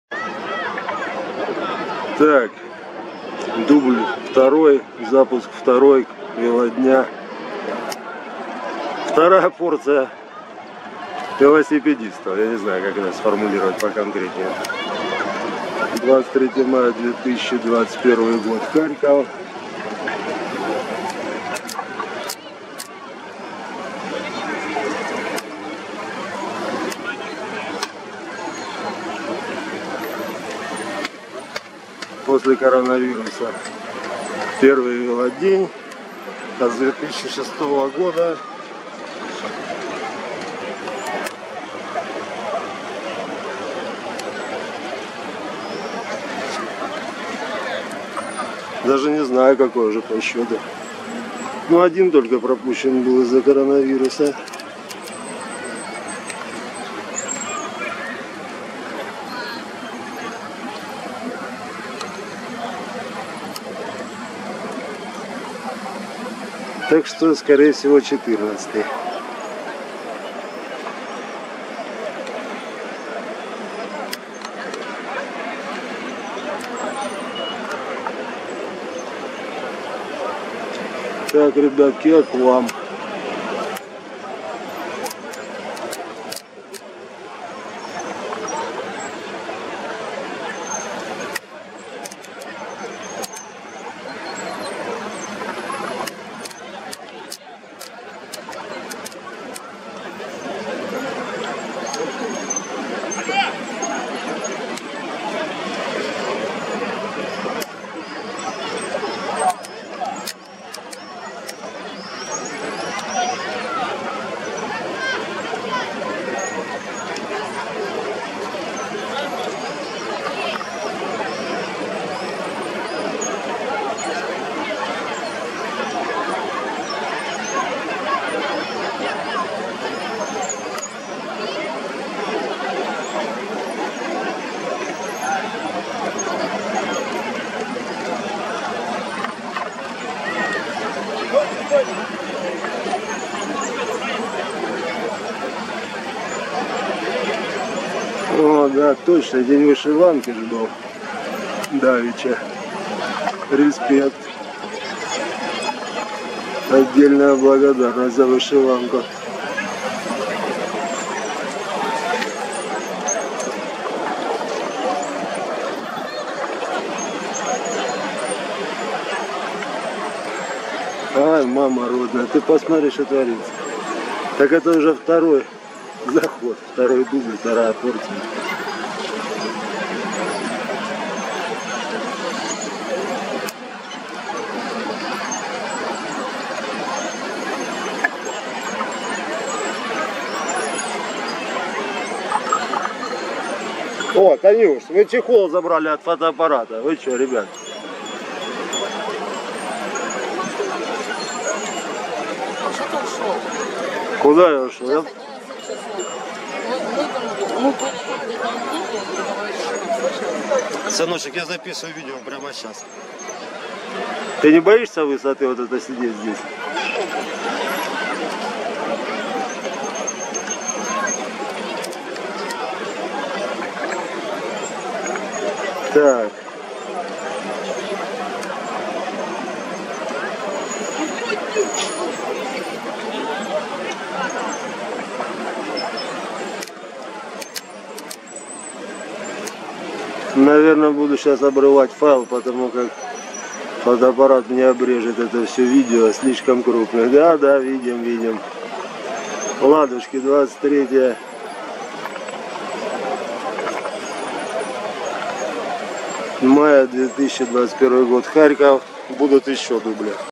Велодень 23 мая 2021 в Харькове - колонна велосипедистов на площади Свободы
Начало Велодня-2021 в Храькове - колонна велосипедистов стартует с площади Свободы у Госпрома 23 мая.